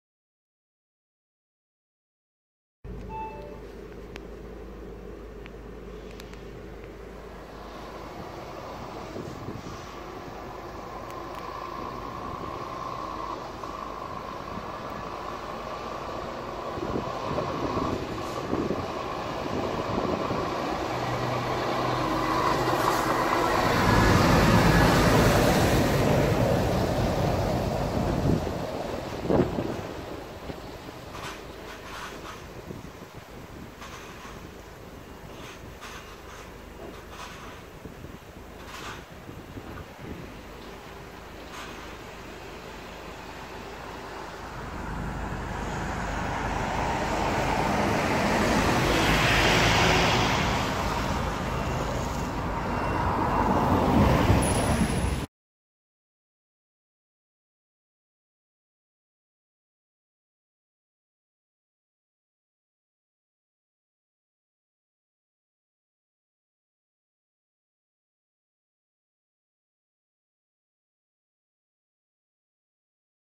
Next Gen Scania R450 ESL MAVIS CYNTHIA H5658 PO68YPA (1-Feb-19)
a Next Gen Scania R450 in Stobart livery
spotted 1st feb 2019 at great bear culina bulwell Nottingham.